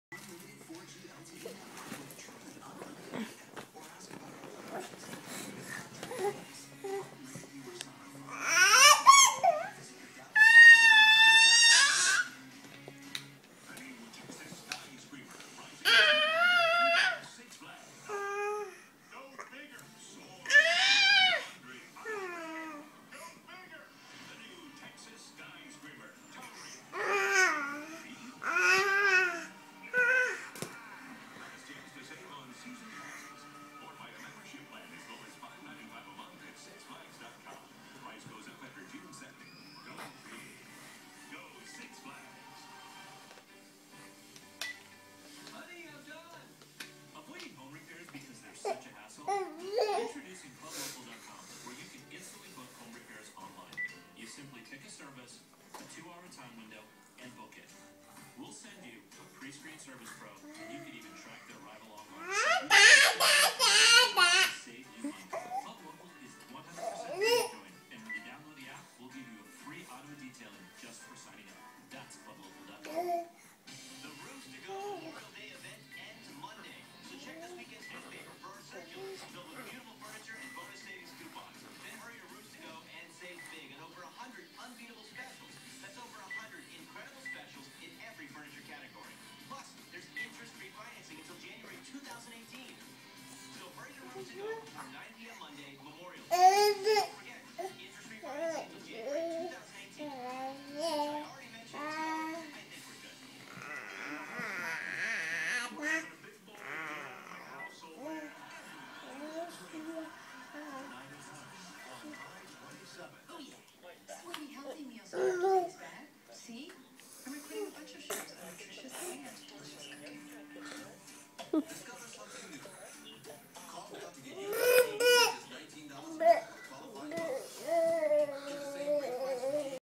10 month old Ethan baby standing and talking on phone
10 month old Ethan baby standing and talking on phone